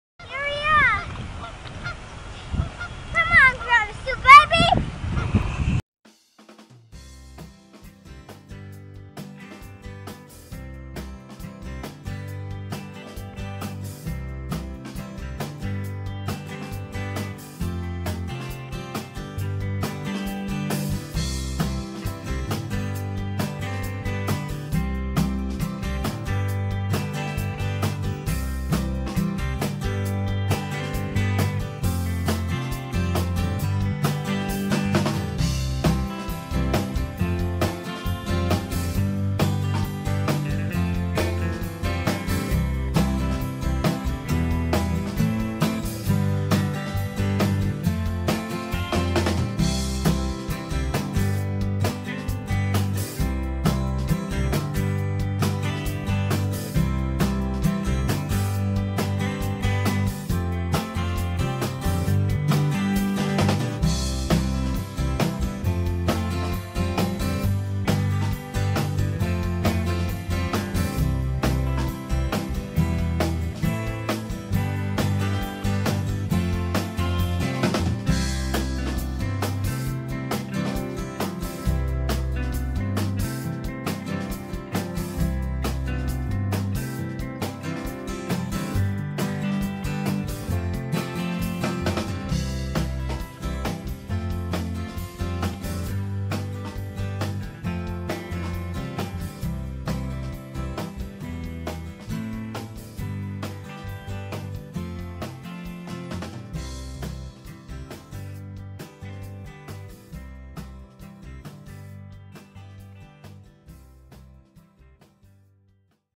Grandma Sue BB #5
Making a basic potato salad. Feel free to experiment with your favorite flavorings, spices, use red potatoes...